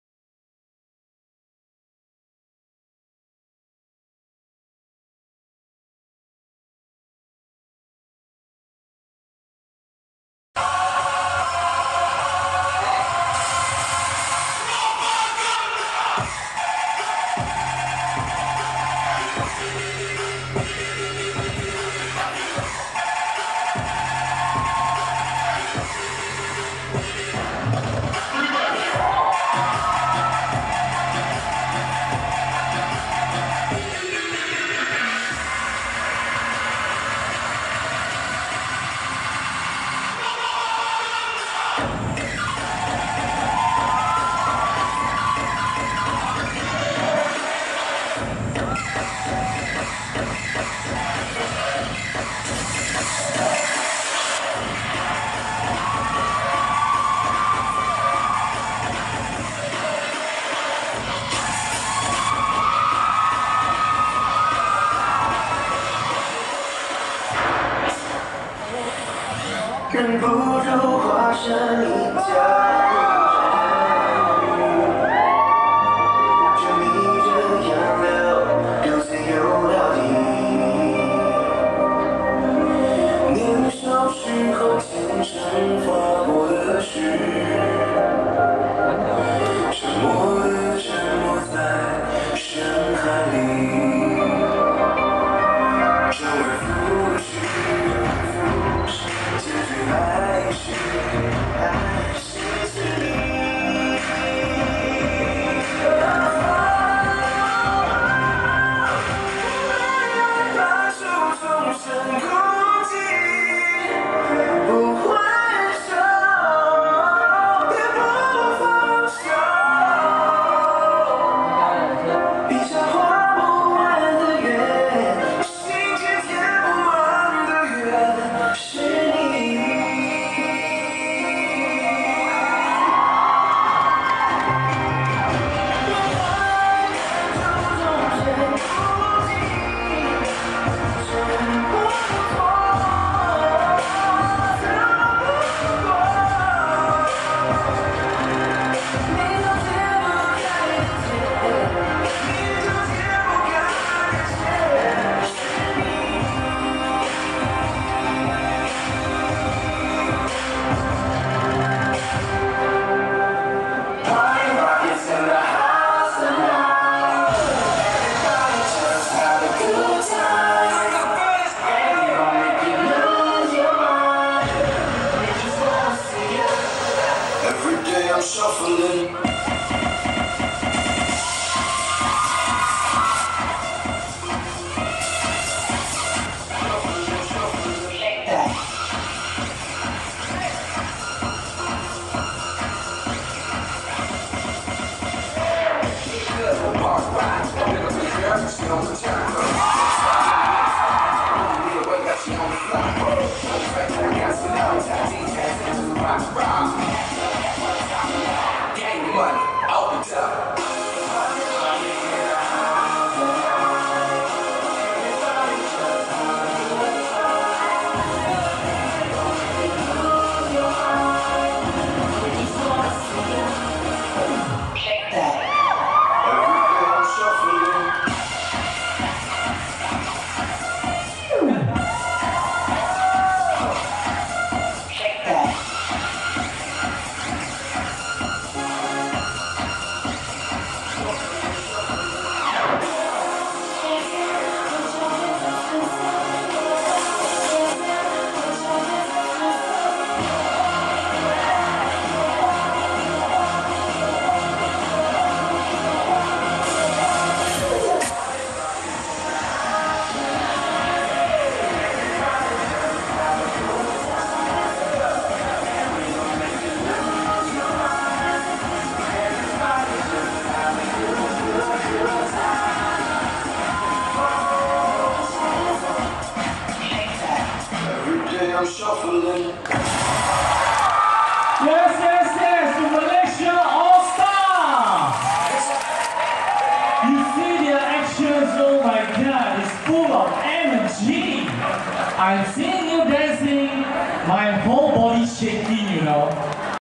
Performance - Street Jazz - by Malaysia All Star Dance Crew
Event - Rebecca Lee's Dance Party 2017
Theme - Hollywood Movie